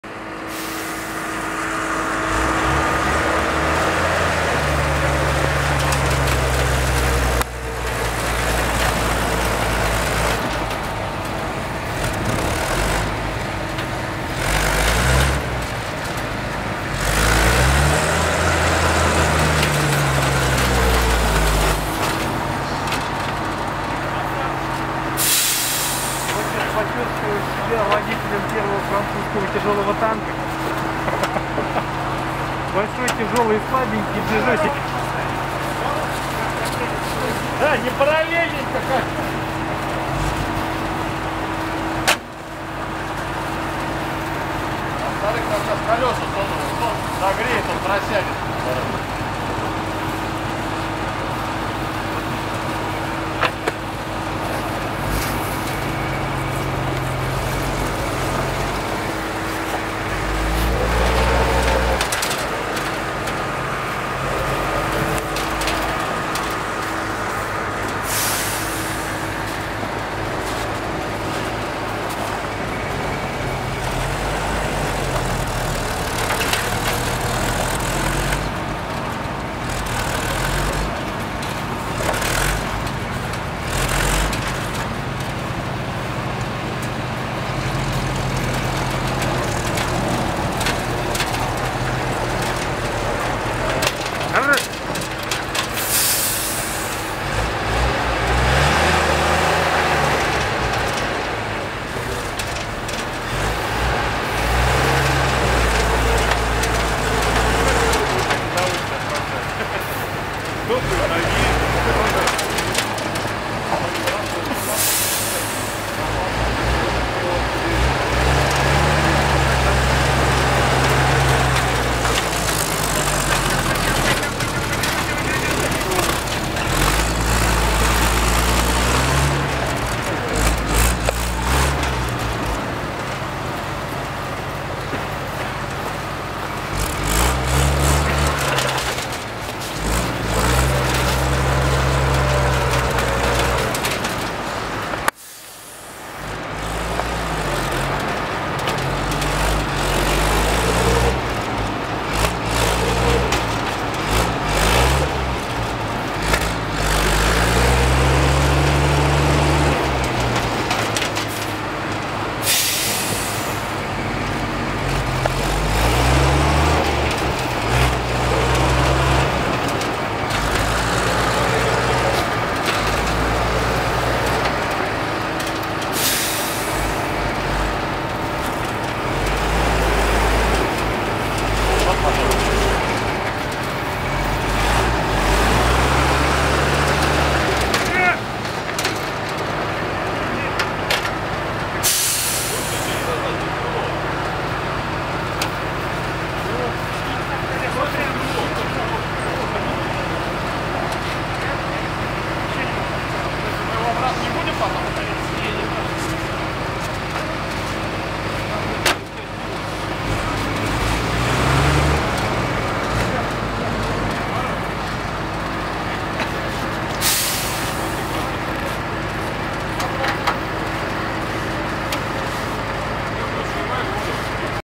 ЗиС-150
Перегон ЗиС-150 от Ил-14 под крыло Ан-26  на Тушинском аэродроме. 29 января 2012г.